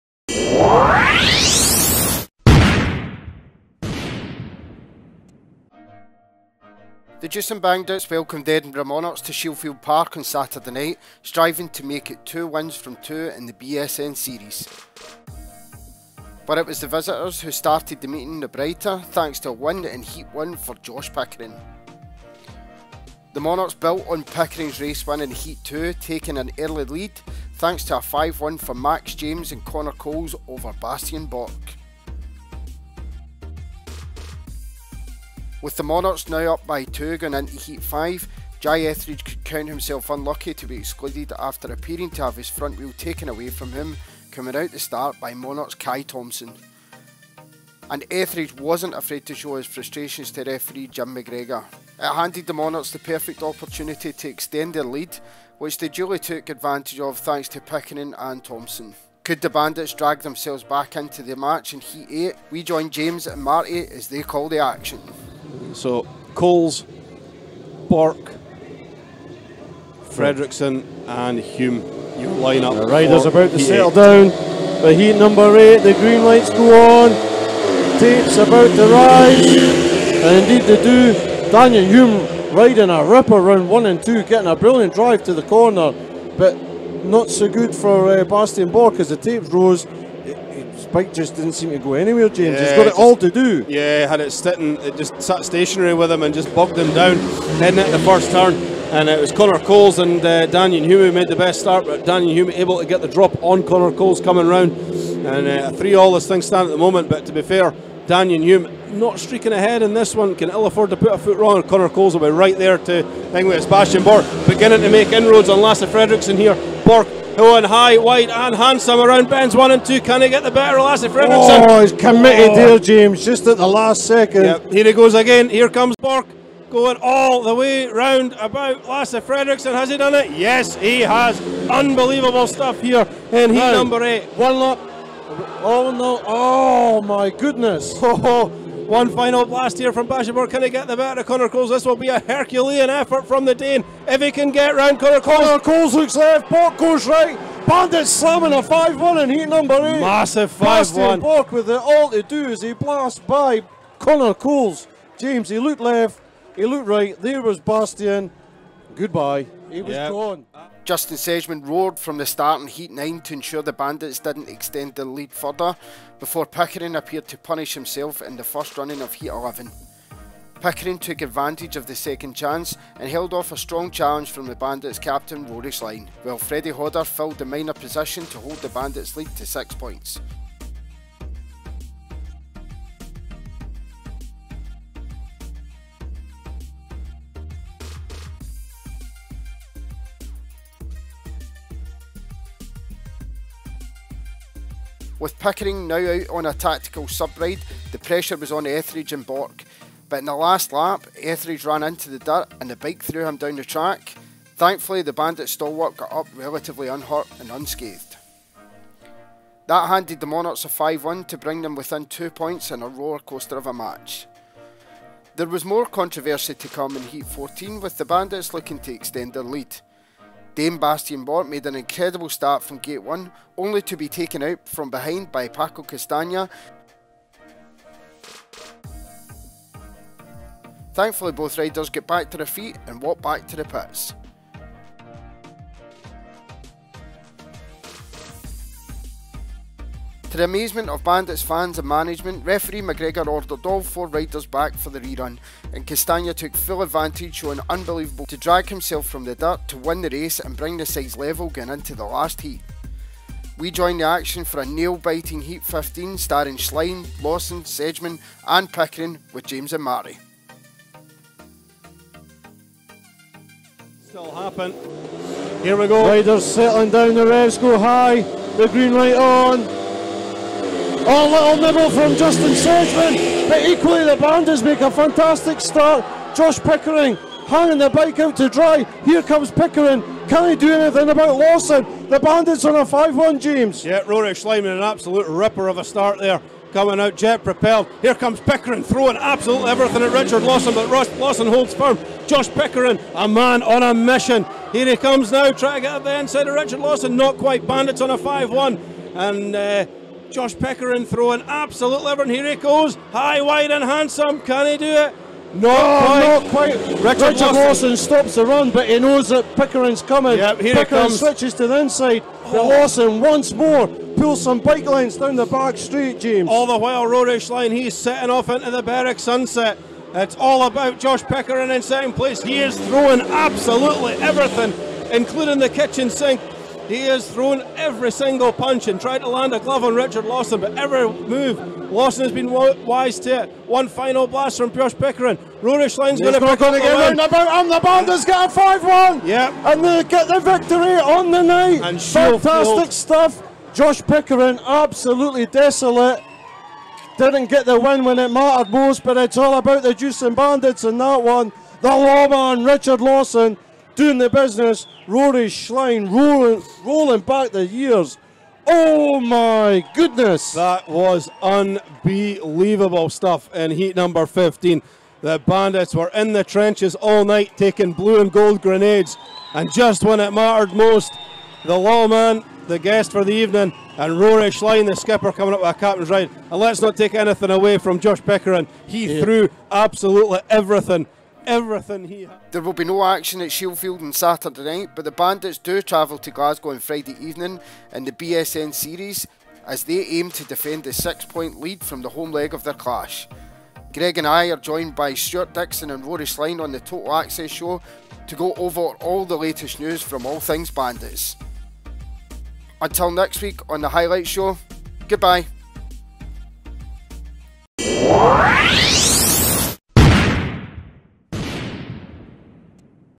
𝙏𝙊𝙏𝘼𝙇 𝘼𝘾𝘾𝙀𝙎𝙎 - 𝙈𝙊𝙉𝘼𝙍𝘾𝙃𝙎 𝙃𝙄𝙂𝙃𝙇𝙄𝙂𝙃𝙏𝙎
The Jewson Bandits took on the Edinburgh Monarchs on Saturday evening at Shielfield Park. Check out the best of the action from a nail biting clash that went all the way to a last heat decider.